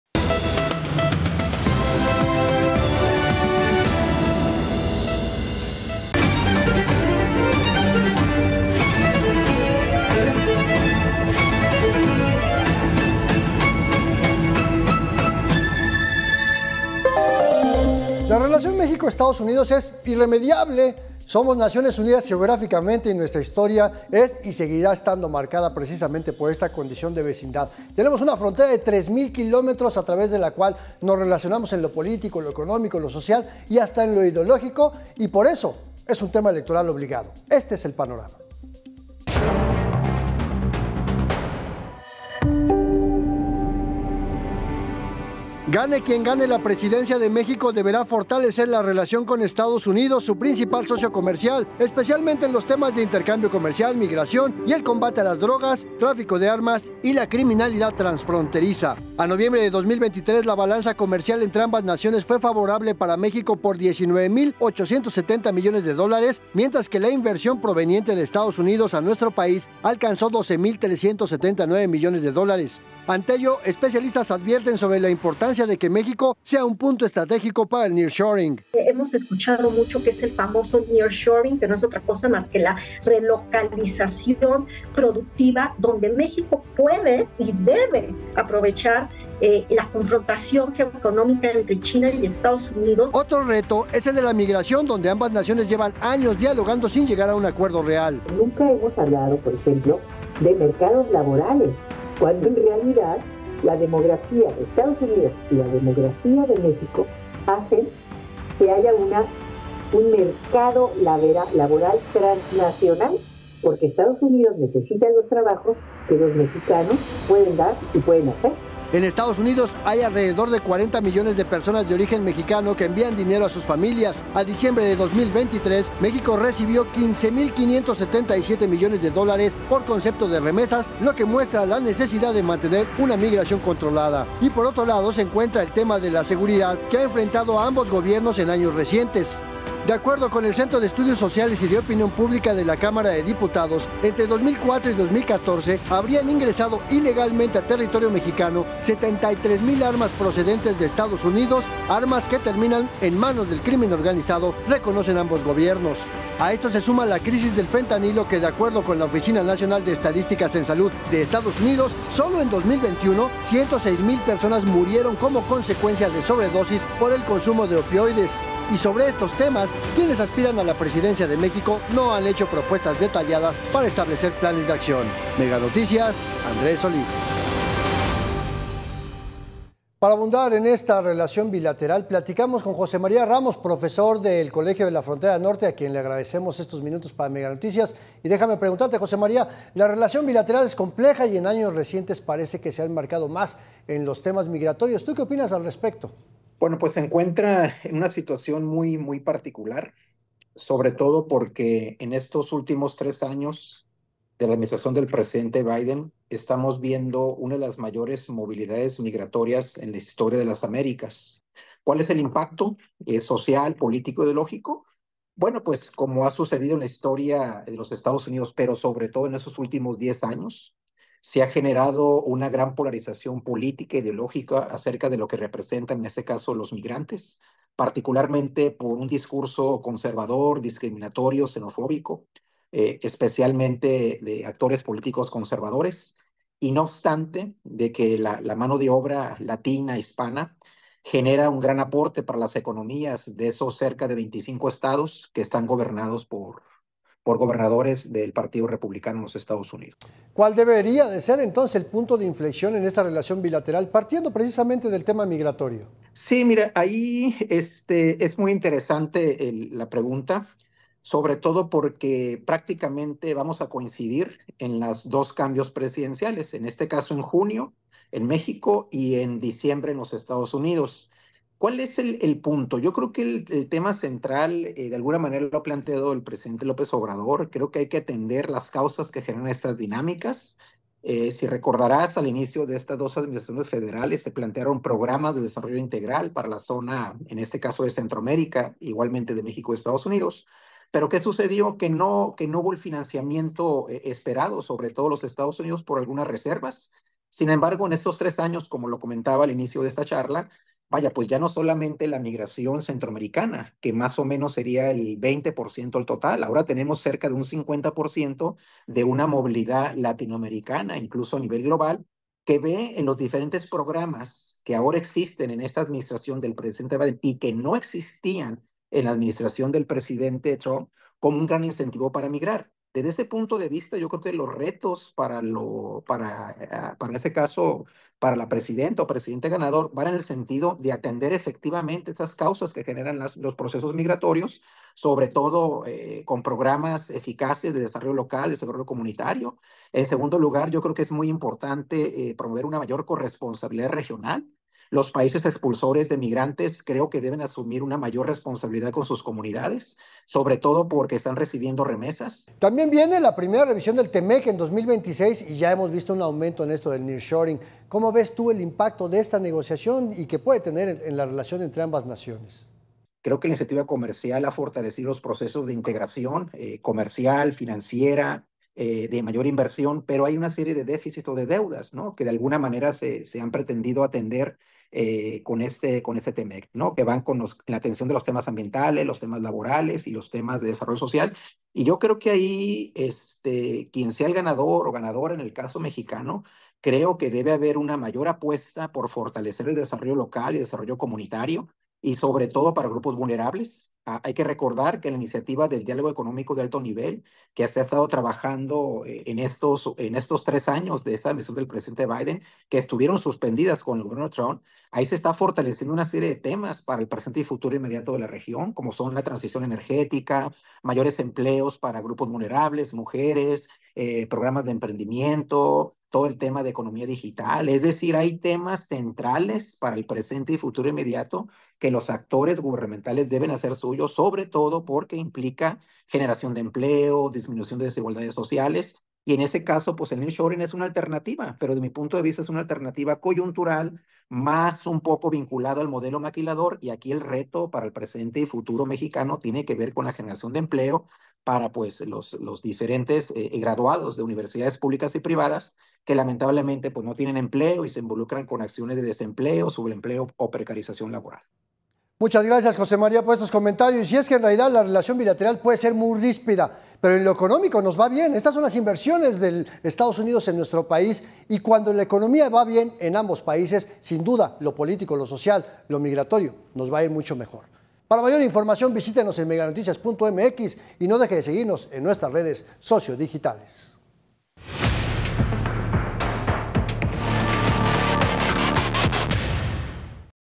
01 CAPSULA APUESTAS EN LA RELACION BILATERAL
México y Estados Unidos comparten tres mil kilómetros de frontera, además de 200 años de historia compartida y una relación basada en el estira y afloja.